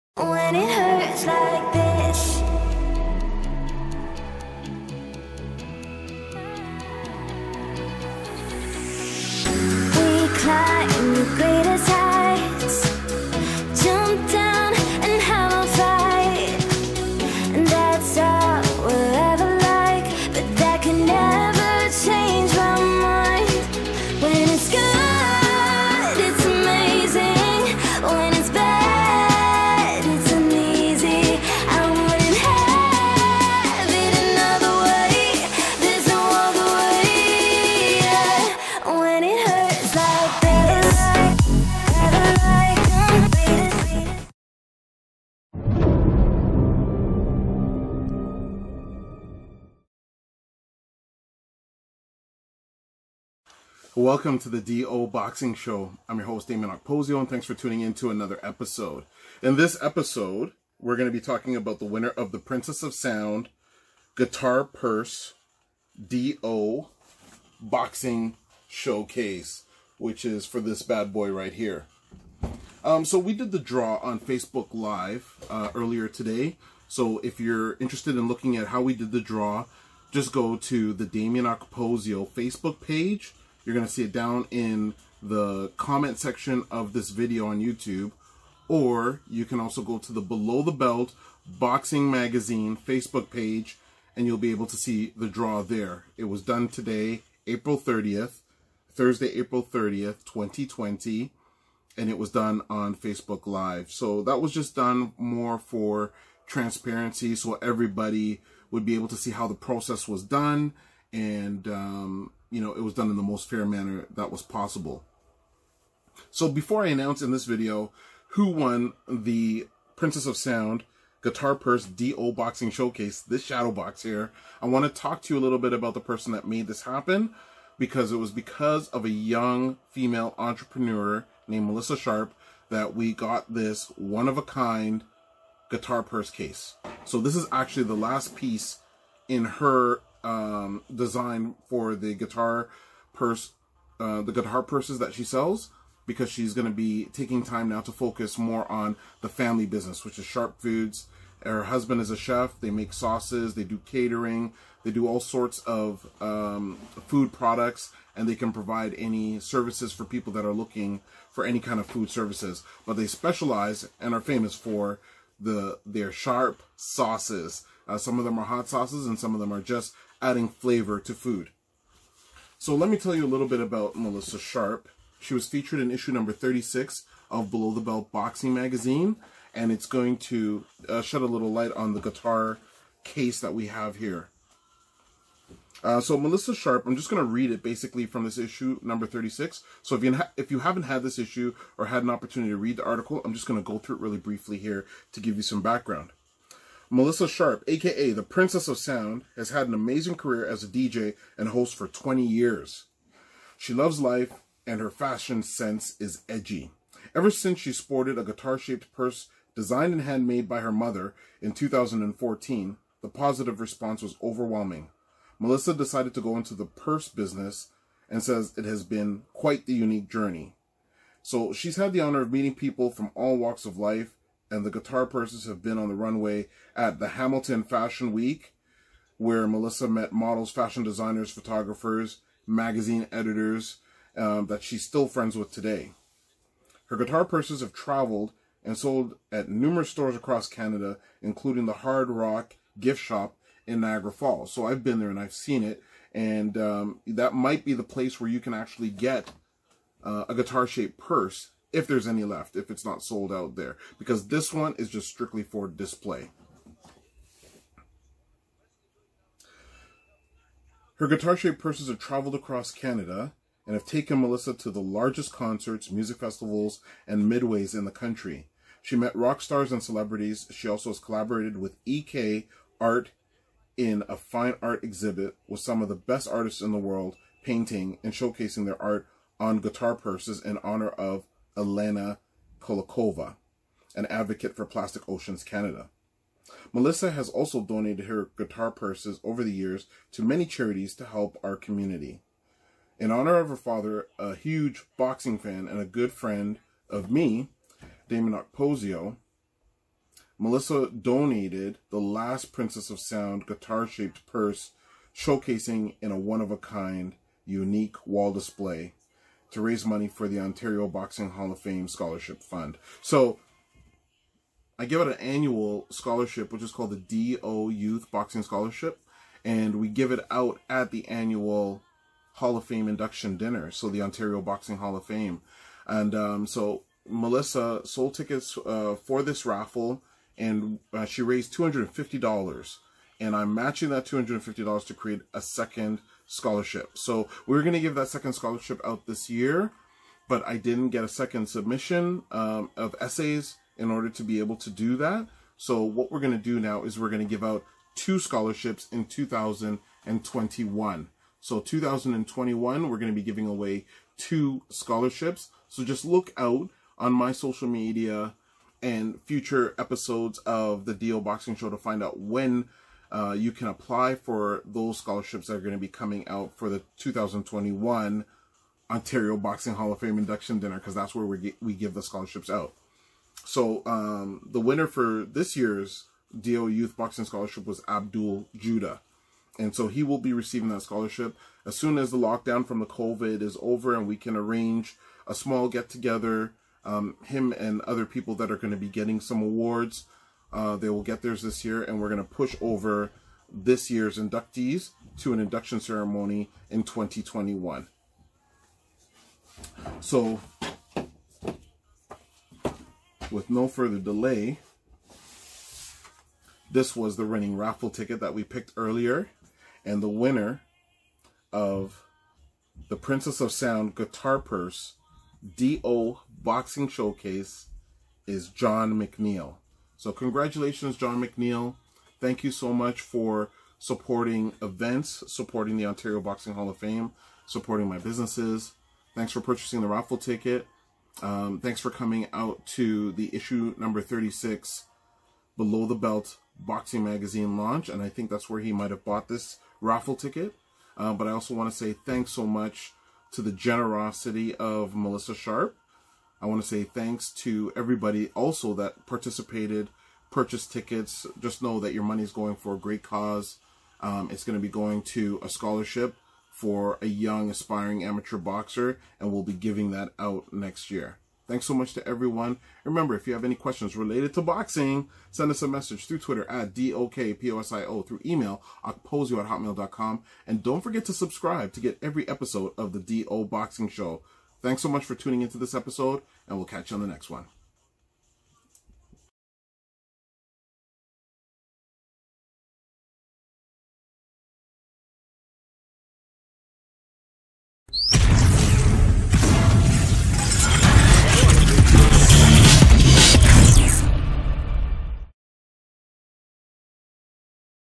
Guitar Purse Display Winner - Episode 735 - DO Boxing Show
On Thursday, April 30, 2020, we announced the winner of the "Princess of Sound Guitar Purse DO Boxing Showcase". We raised $250.00 thanks to the generosity of entrepreneur Melissa Sharpe, and all who purchased tickets. The proceeds will be matched by Dameon Okposio to create a second $500 scholarship for the annual DO Youth Boxing Scholarship in 2021. Many thanks to our supporters for the continued faith and support.